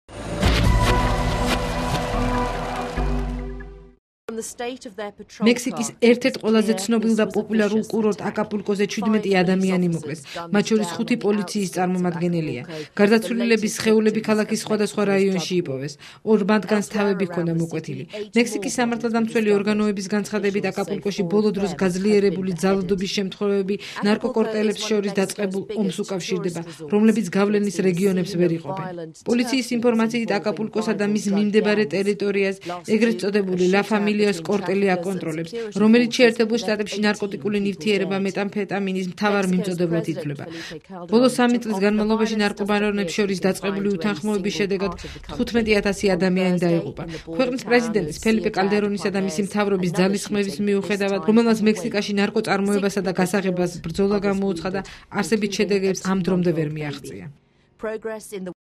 14/03/2010
მექსიკაში ნარკობარონებს შორის მიმდინარე დაპირისპირების შედეგად 17 ადამიანი დაიღუპა